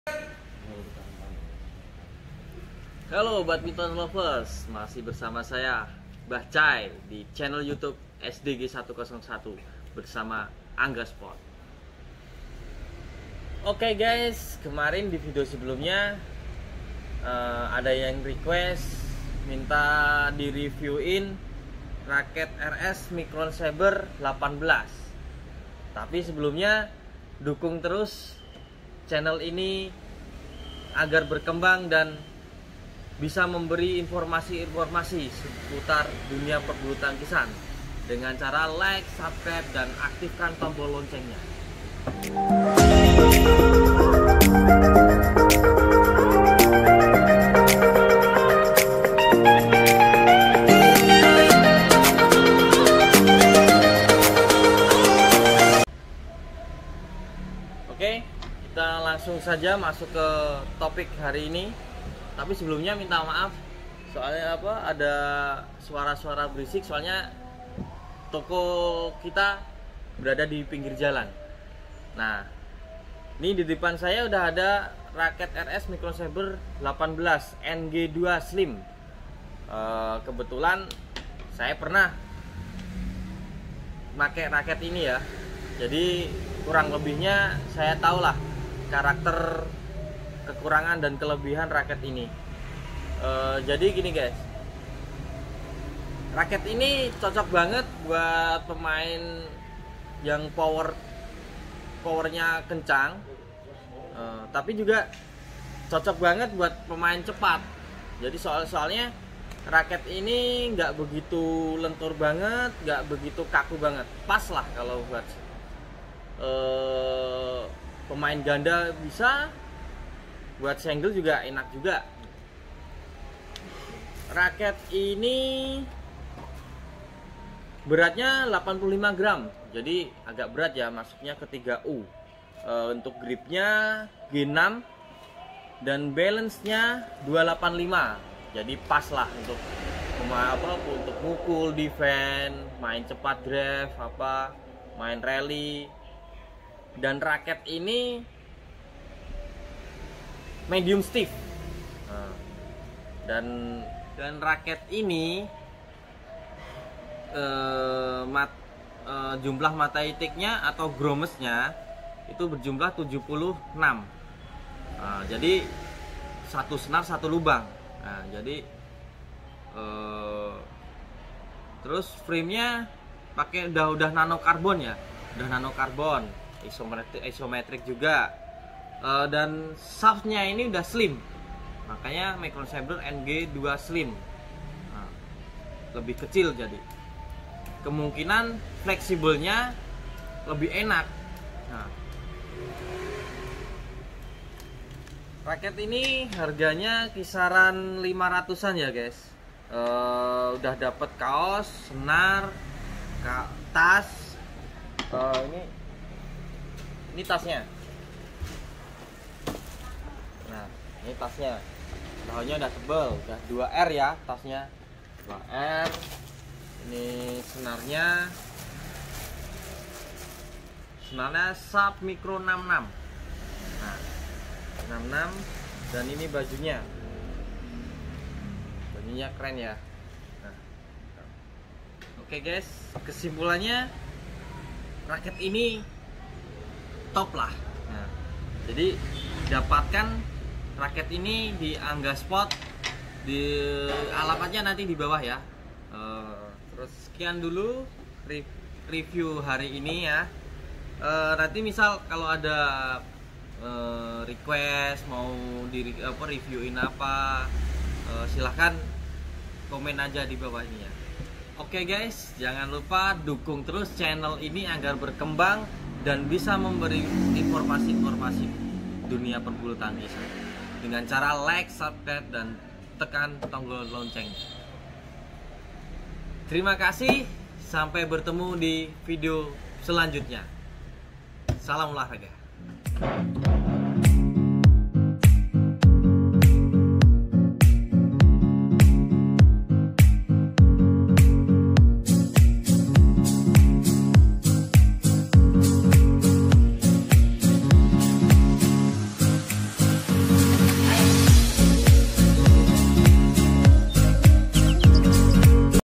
Reinforce Speed MS 18 NG2 Slim | Murah tapi gak murahan | ANGGA SPORT PATI
Review raket REINFORCE MS18 NG2 SLIM
untuk pembelian bisa langsung ke toko ANGGA SPORT PATI  beralamatkan di Jl.Dr.Susanto No 55 Prenggan Pati
Atau bisa juga secara online dengan chat WA nomer fi bawah ini ya.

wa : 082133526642